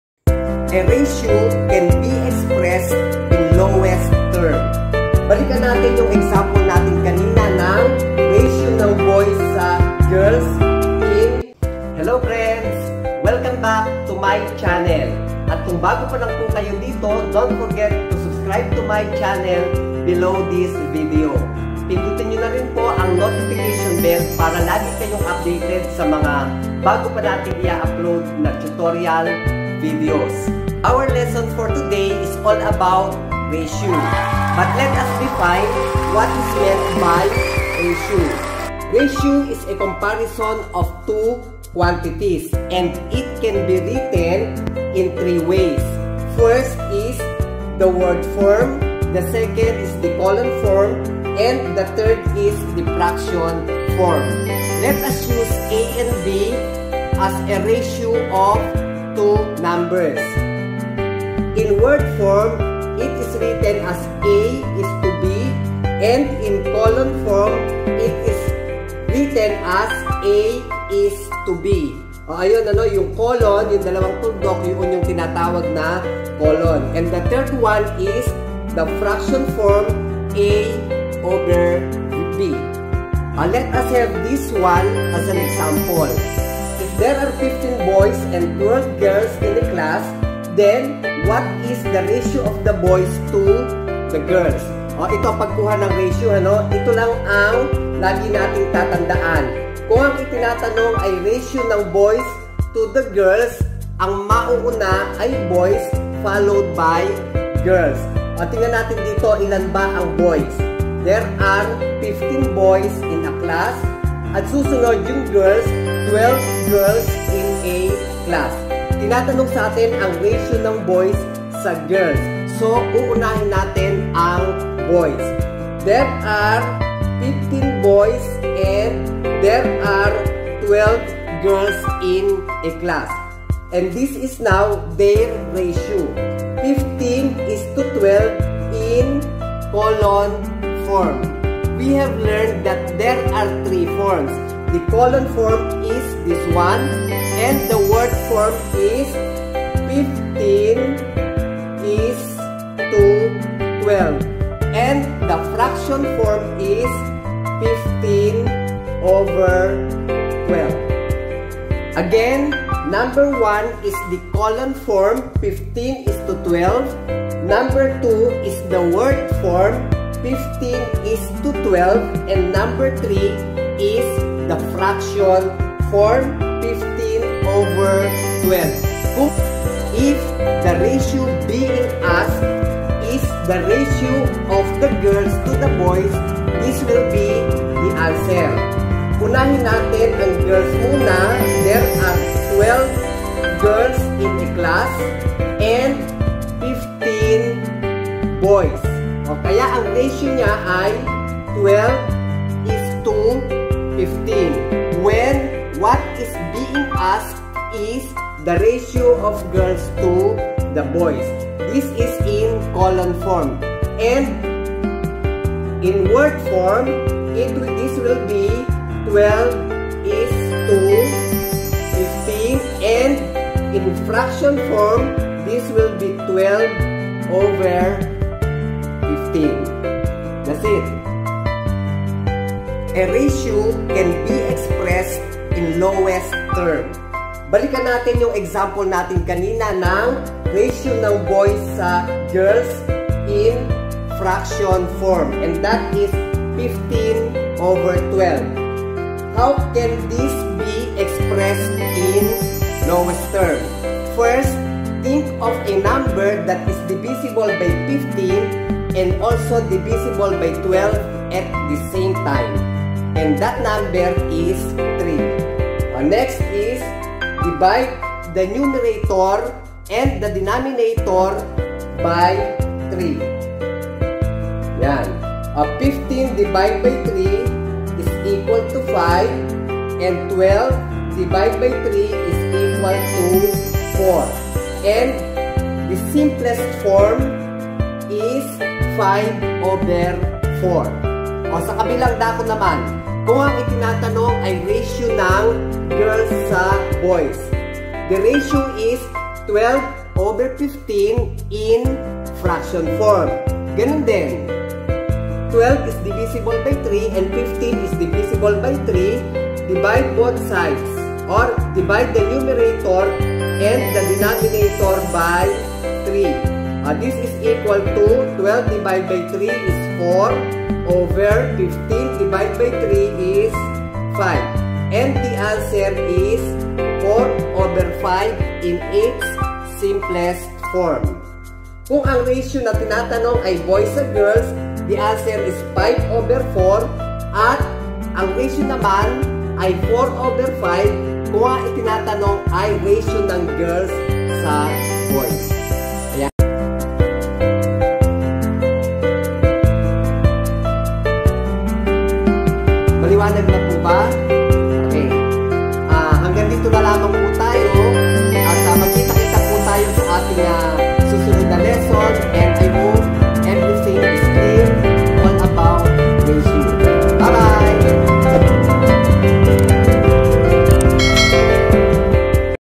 CONCEPT OF RATIO ( a : b ), ( a is to b ), ( a / b ), MATH 6, QUARTER 2, WEEK 1 billy de belen
CONCEPT OF RATIO ( a : b ), ( a is to b ), ( a / b ), MATH 6, QUARTER 2, WEEK 1 COLON FORM, WORD FORM, FRACTION FORM,  LOWEST TERM DIVISIBILITY MATHinik billy de belen